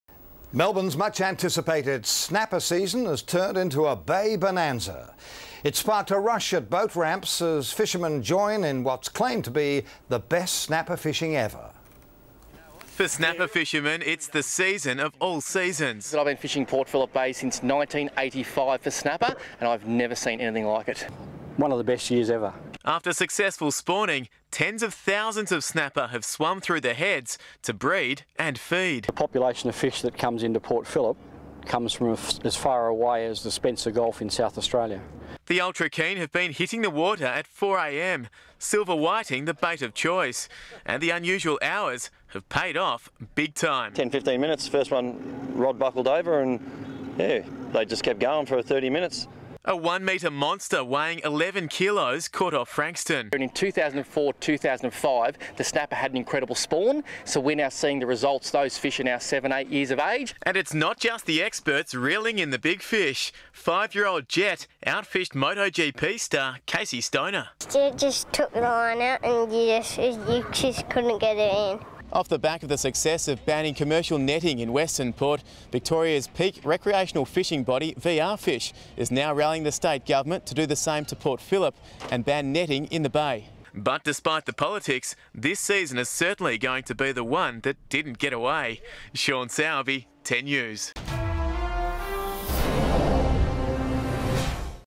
What a Catch
Melbourne's much anticipated snapper season has turned into a Port Phillip Bay bonanza.